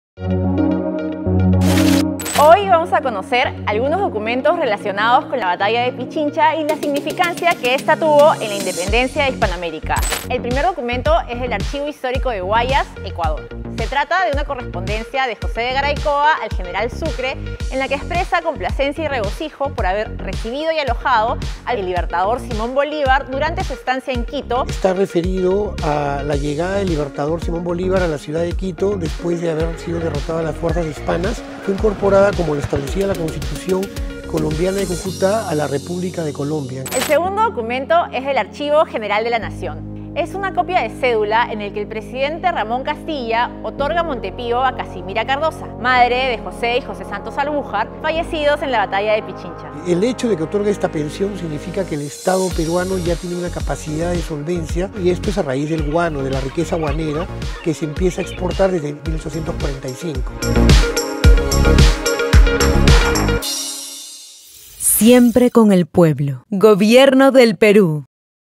El Documento del Mes - Resumen Nº 19
Acompáñanos a conocer el episodio N° 19 de “El documento del mes”. En esta ocasión te presentamos documentos relacionados a la batalla de Pichincha y su significado para la independencia de Hispanoamérica.